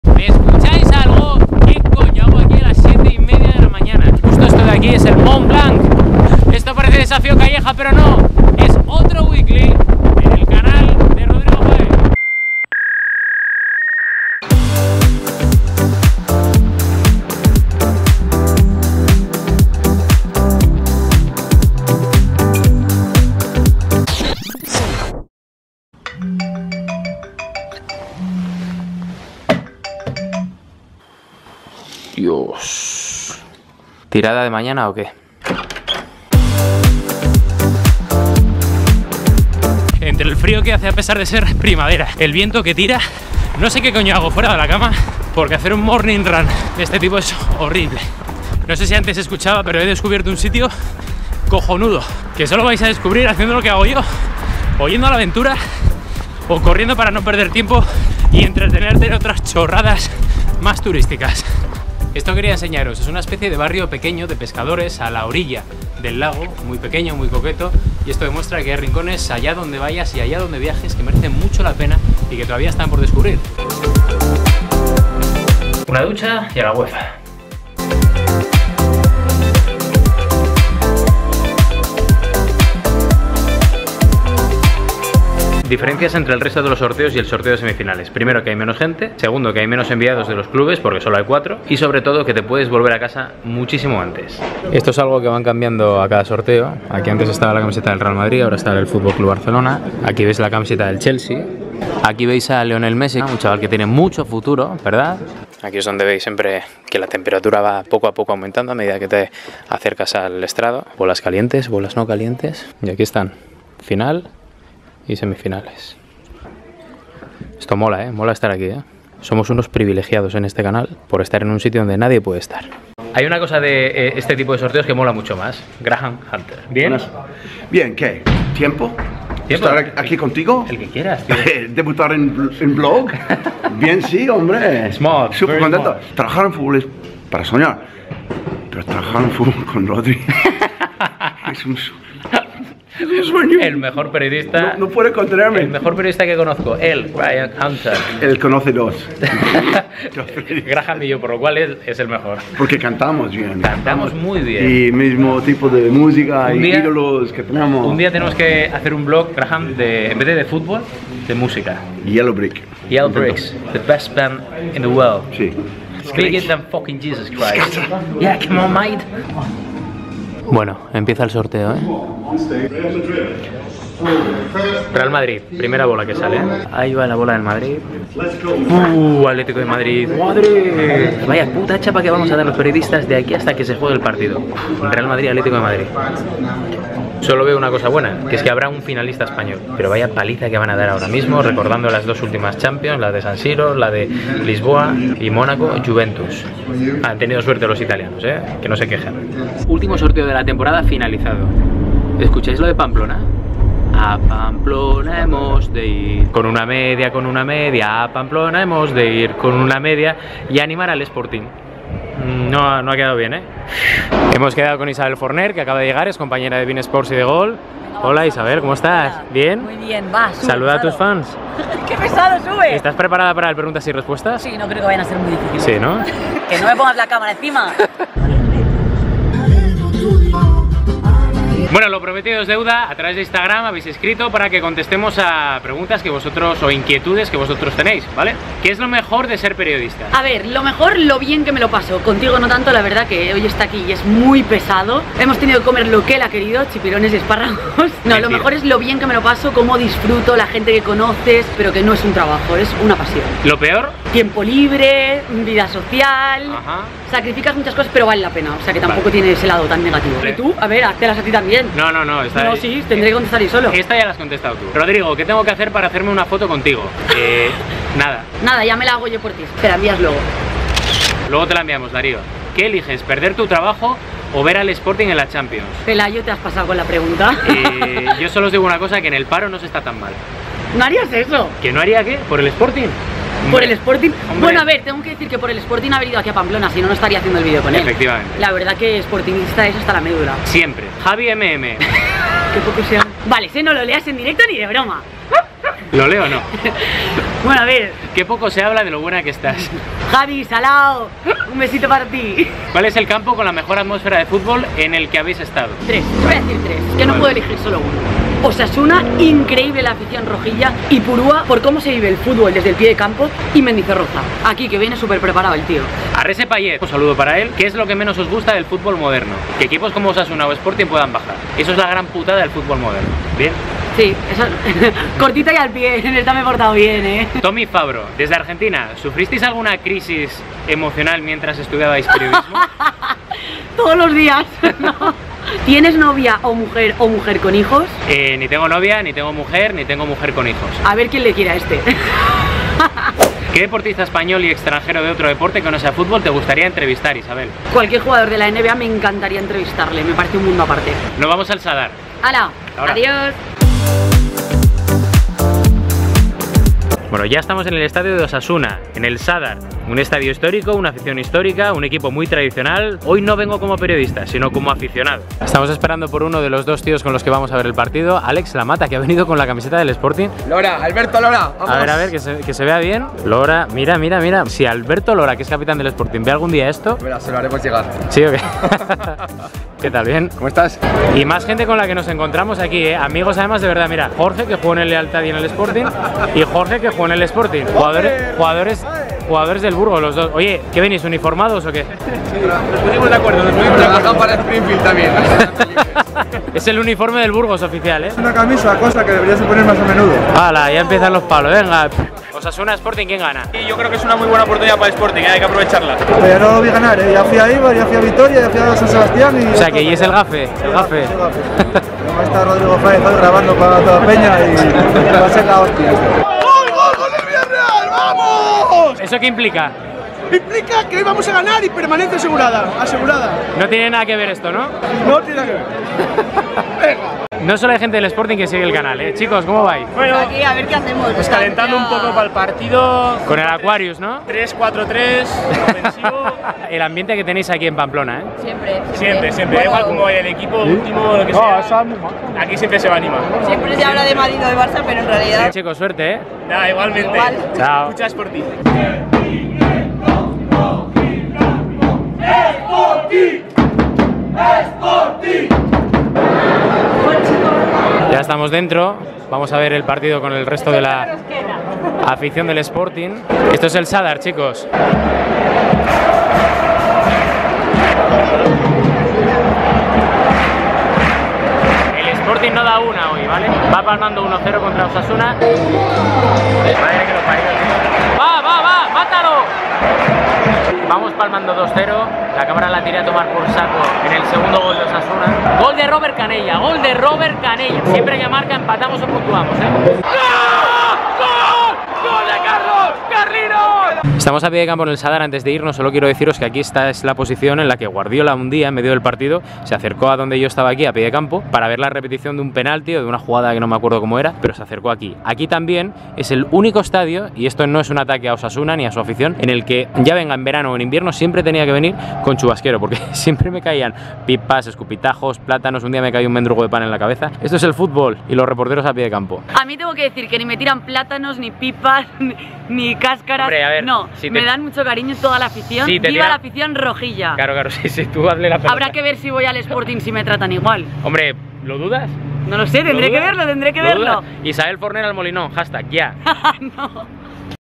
HORROR EN... | Rodrigo Fáez Vlog 19 WEEKLY
¿Sabéis quién es Isabel Forner? Pues en este video la conoceréis porque nos fuimos a Pamplona a ver el Osasuna-Sporting. Antes de eso, nos fuimos al sorteo de las semifinales de la CHAMPIONS LEAGUE a Suiza, al cuartel general de la UEFA... ¿Qué pasó ahí? Lo veréis en el video... XD